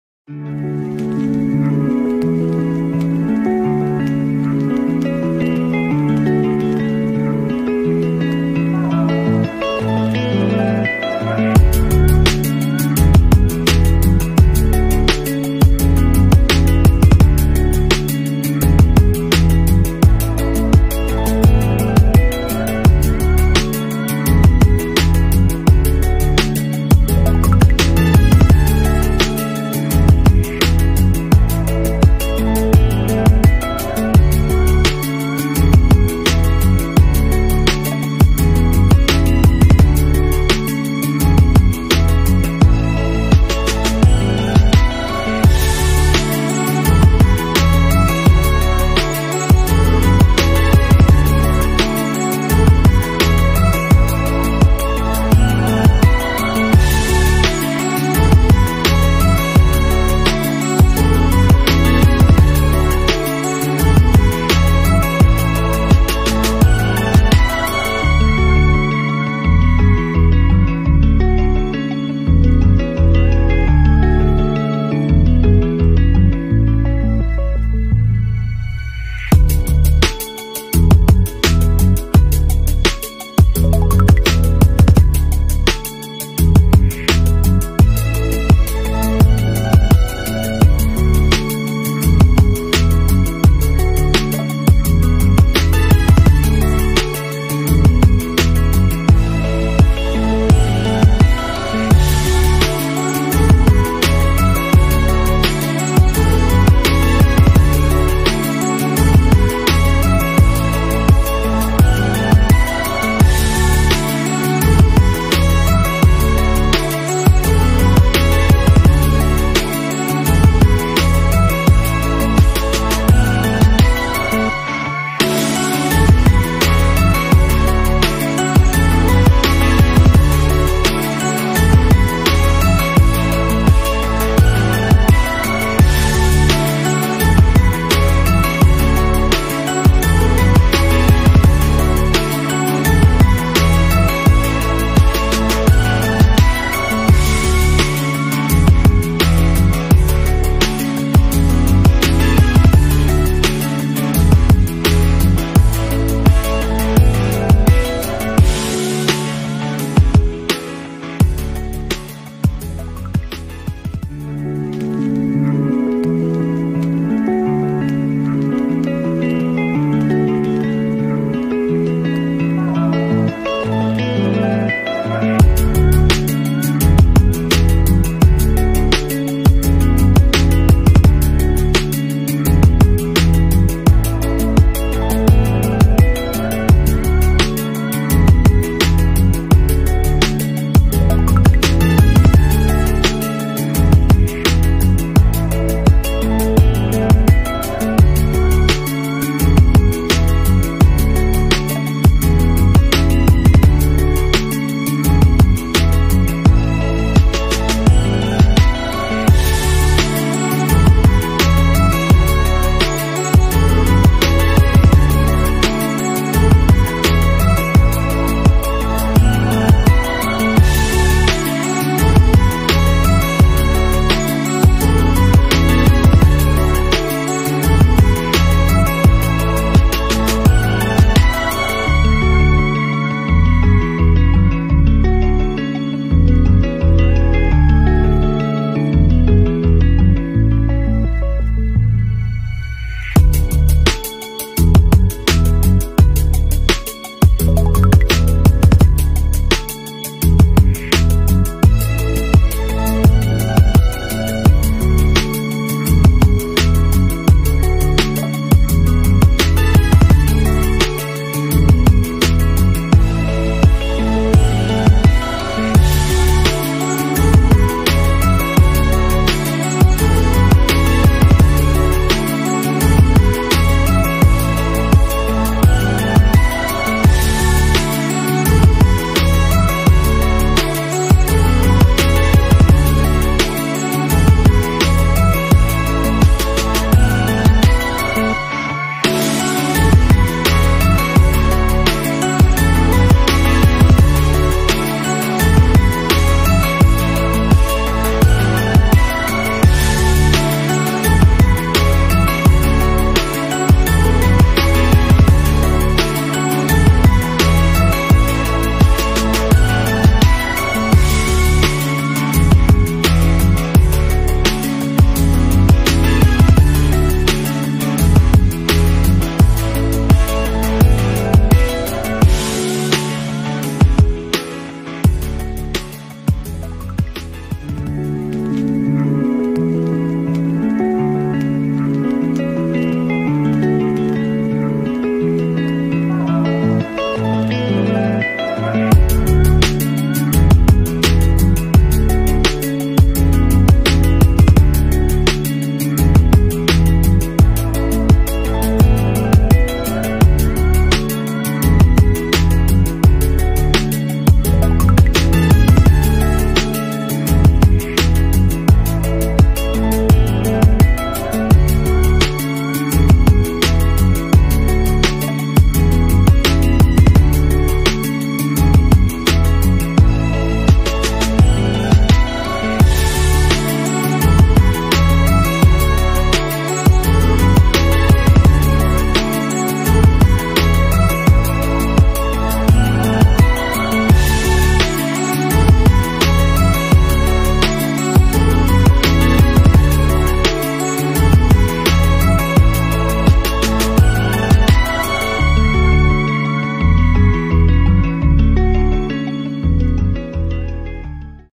হালিম ছাড়া ইফতার চলে? Easy recipe of delicious chicken halim
let's try this recipe with ready-made halim mix and give surprises to your closed ones. Tgis revipe is simple, following each and every steps will give you the best result. Trust me.